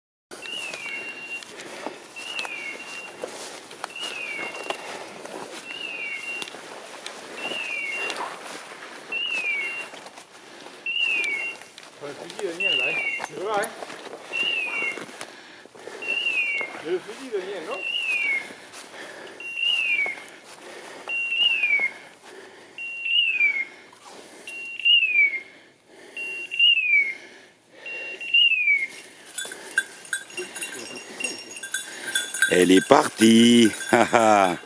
Une vieille maline qui ne se laissera pas prendre dans l'objectif.Les chiennes la retrouveront trois fois, mais impossible à voir. La troisième fois Lady nous envoie un wouah wouah, signe qu'elle lui est parti devant